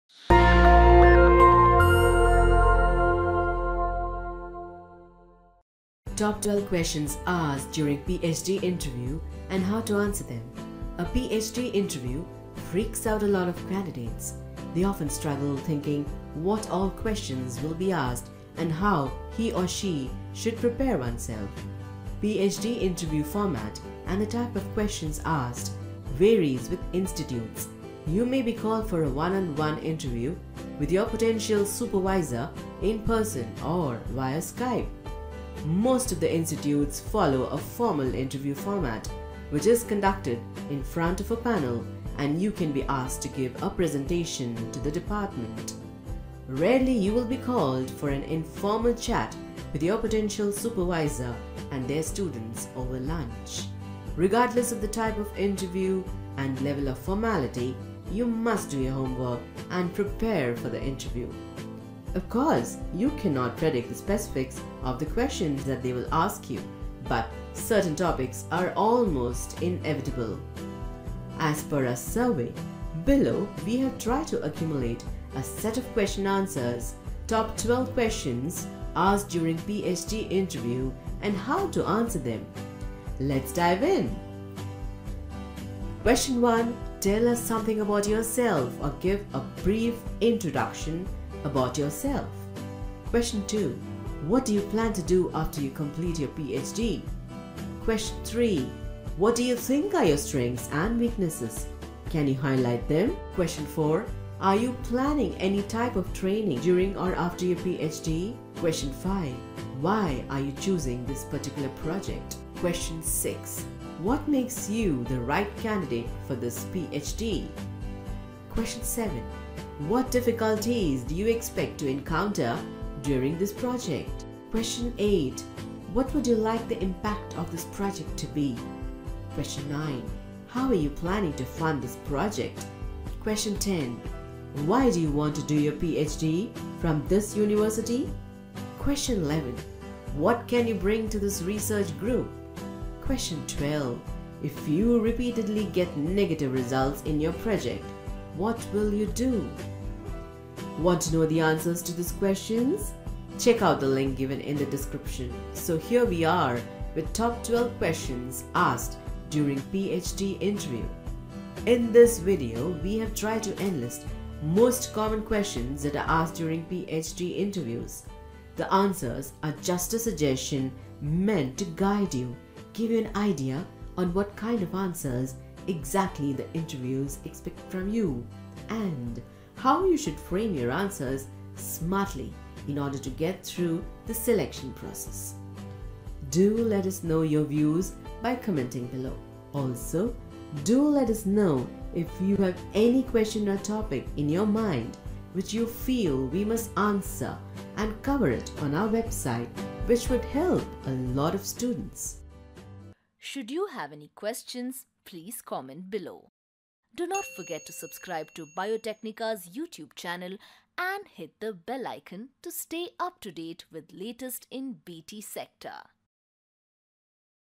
Top 12 Questions Asked During PhD Interview & How to Answer Them
A Ph.D. Interview freaks out a lot of candidates. They often struggle thinking what all questions will be asked and how he or she should prepare oneself.

PhD Interview format and the type of questions asked, varies with institutes. You may be called for a one-on-one interview with your potential supervisor in person or via Skype.

Most of the institutes follow a formal interview format which is conducted in front of a panel, and you can be asked to give a presentation to the department.